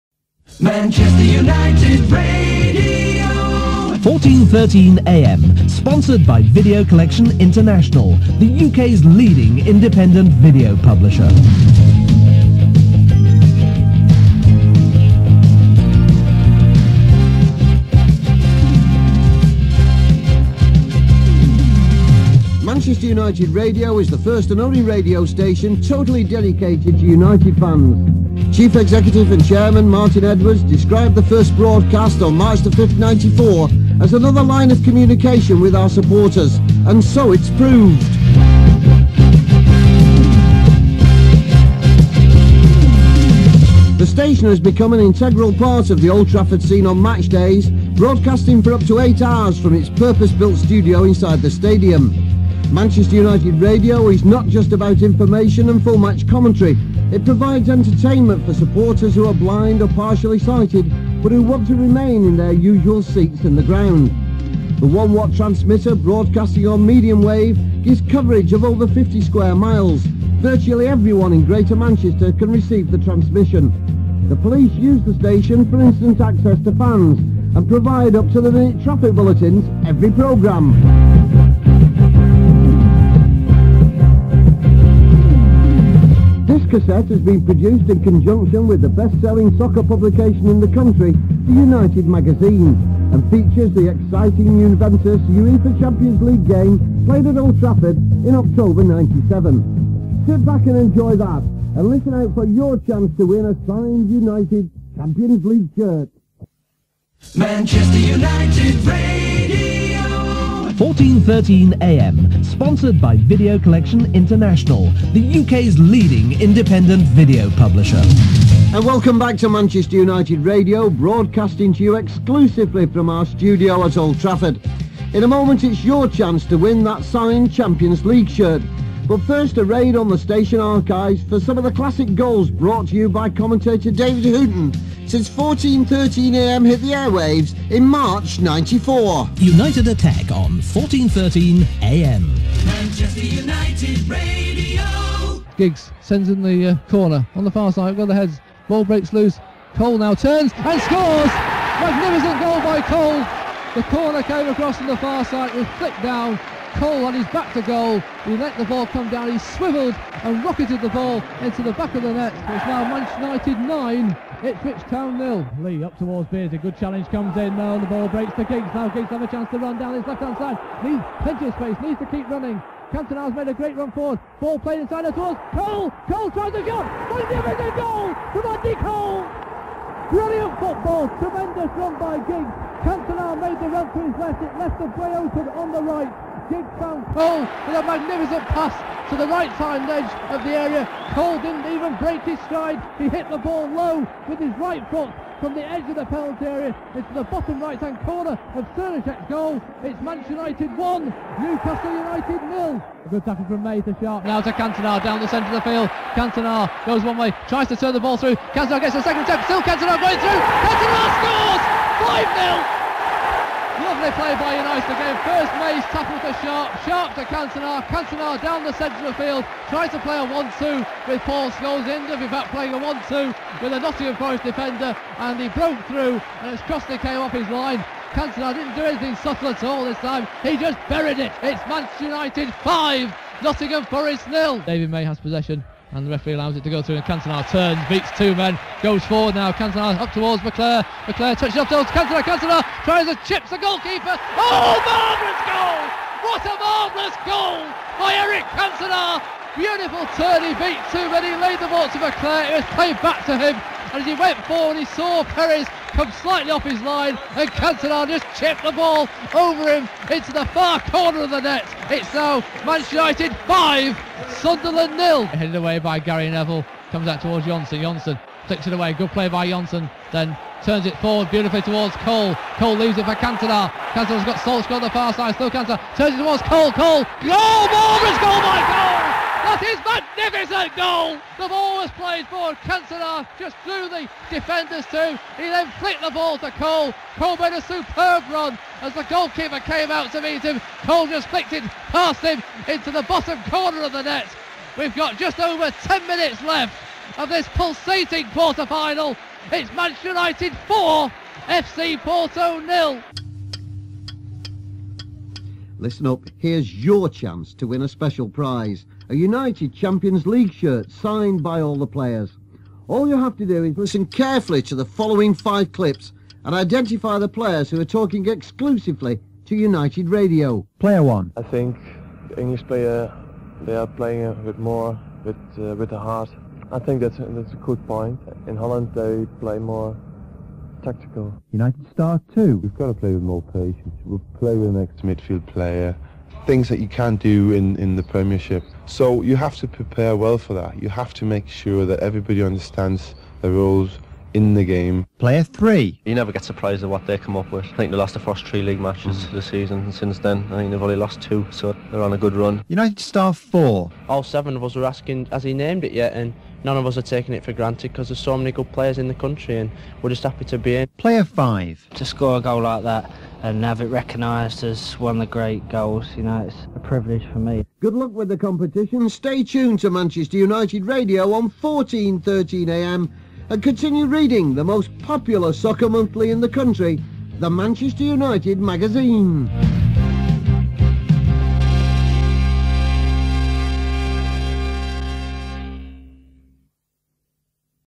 MUFC on Tape - Manchester United Magazine Cassette
A recording of a cassette that came with the Manchester United Magazine's 1997 yearbook. Sadly the audio quality isn't great in places and the commentary segment from Side 1 was so unintelligible that all I could do was cut it out. Still, I thought some people might enjoy hearing it.

I've also put up a bigger version of the cardboard slip it came in, if anyone wants a closer look